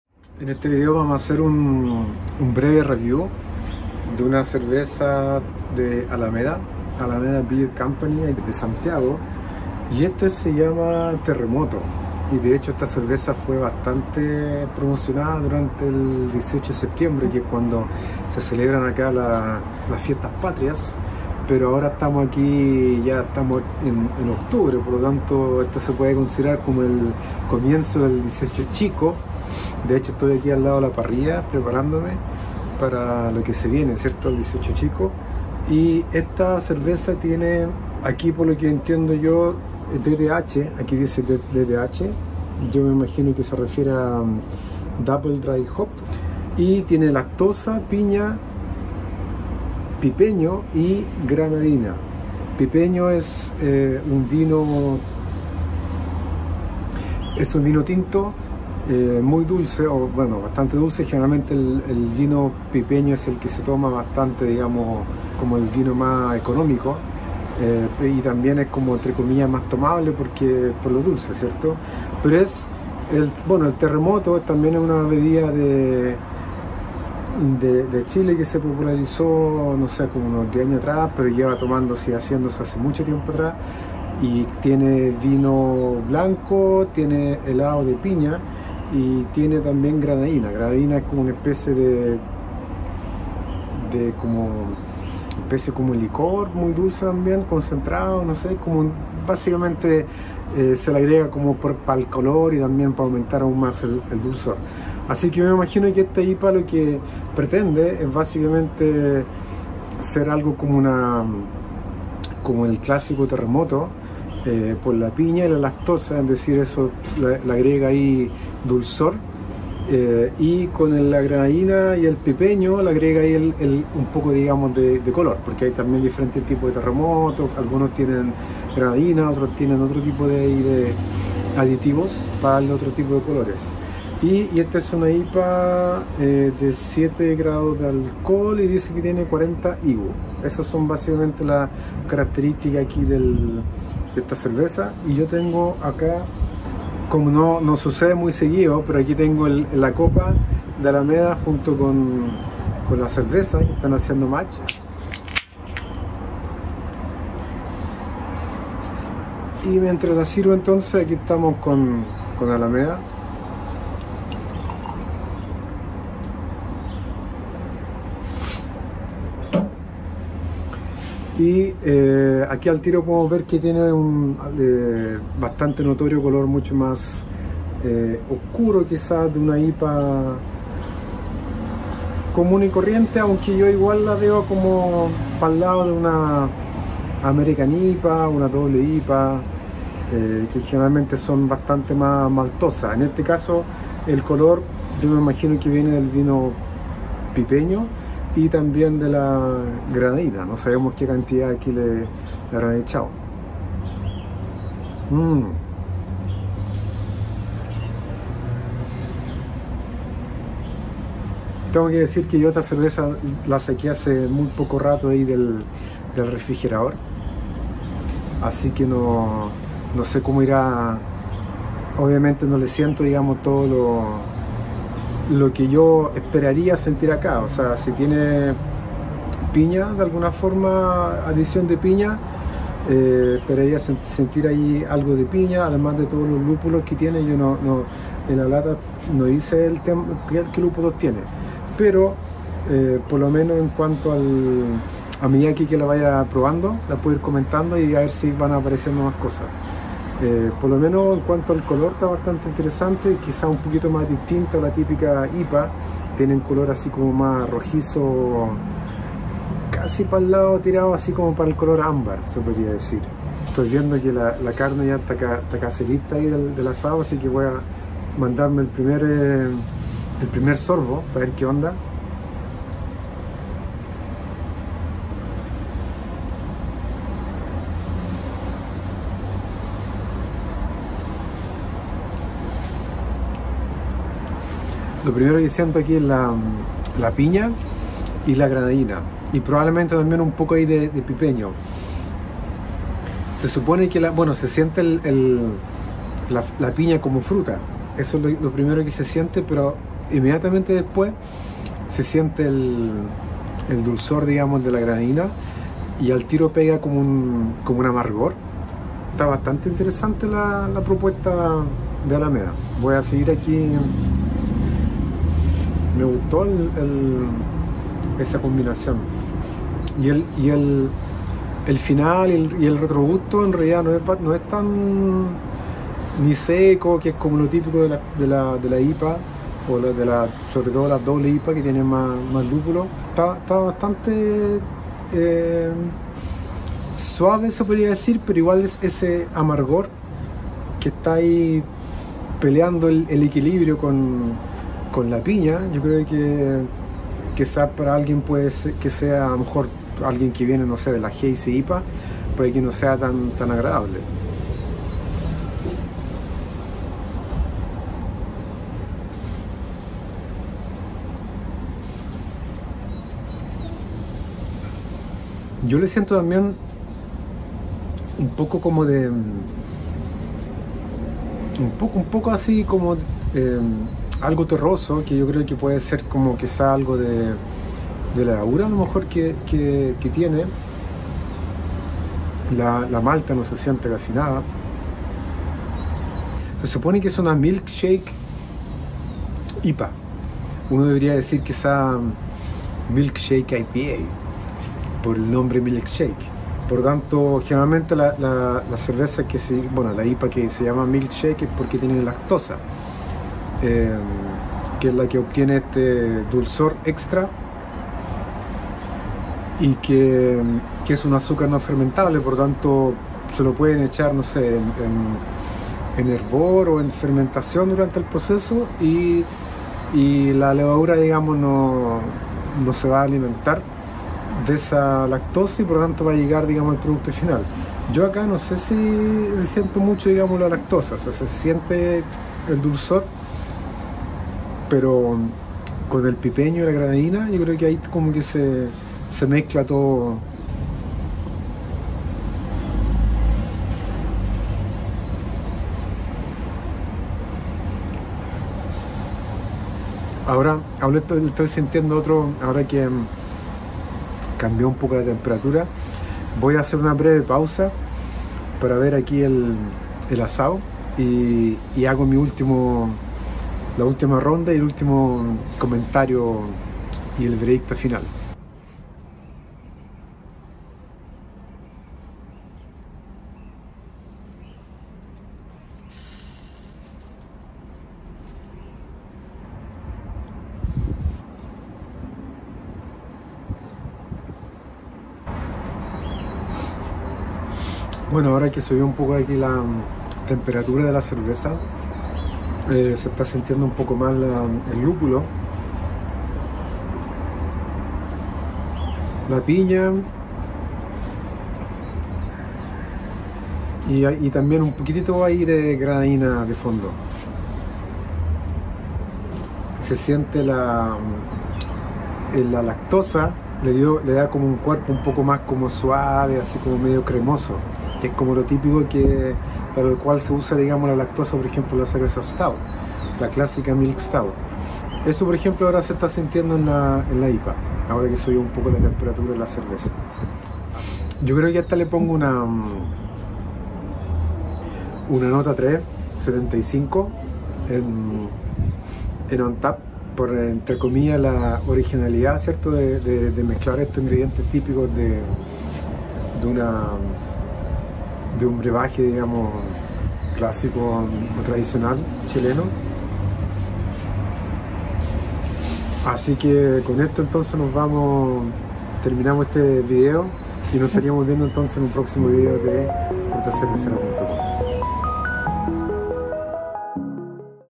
Beer Review - Terremoto (Milkshake IPA, 7.5% ABV) - Alameda Beer Co.
Review de Terremoto Milkshake IPA de Alameda Beer Co. Se trata de una IPA de 7.5% ABV y 70 IBU con adición de piña, lactosa, pipeño y granadina! Una cerveza inspirada en el trago chileno Terremoto.

Untappd: @pobrezuko